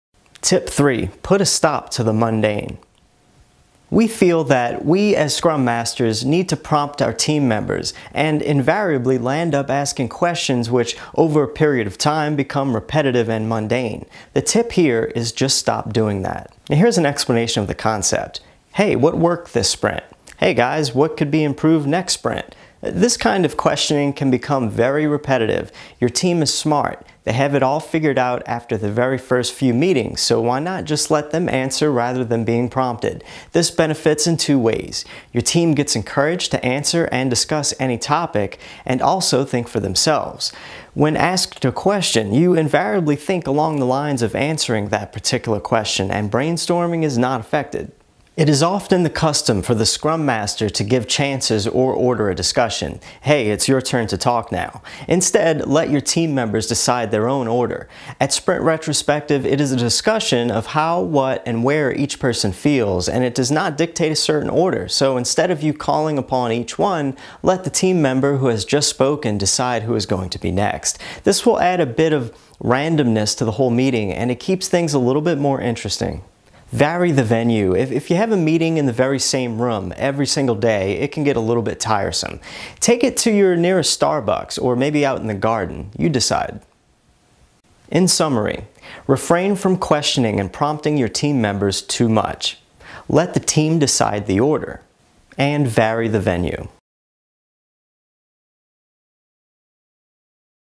Agile Retrospective: Continuous Improvement+Kaizen wth Scrum: Tip 3 – Put a stop to the Mundane
Learn about - Agile Retrospective: Continuous Improvement+Kaizen wth Scrum: Tip 3 – Put a stop to the Mundane - access much more training when you download your daily team checklist using the link above